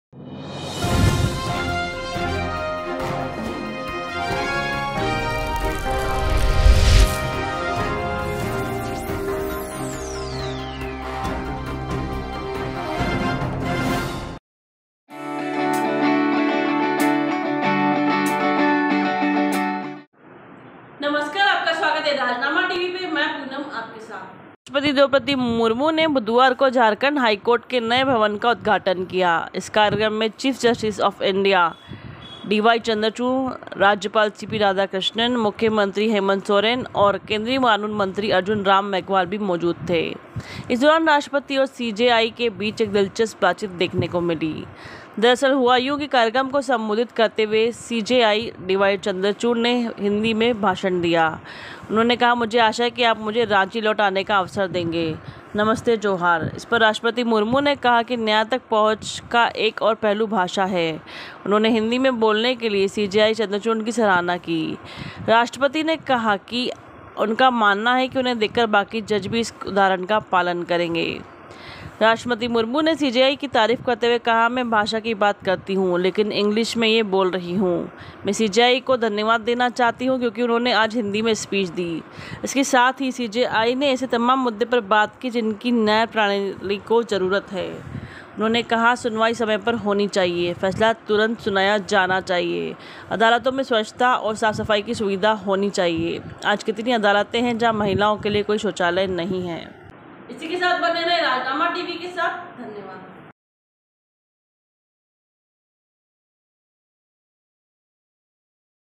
anchorpoonam हिंदी में स्पीच देने पर राष्ट्रपति द्रौपदी मुर्मू ने CJI की तारीफ की
About this video:हिंदी में स्पीच देने पर राष्ट्रपति द्रौपदी मुर्मू ने CJI की तारीफ की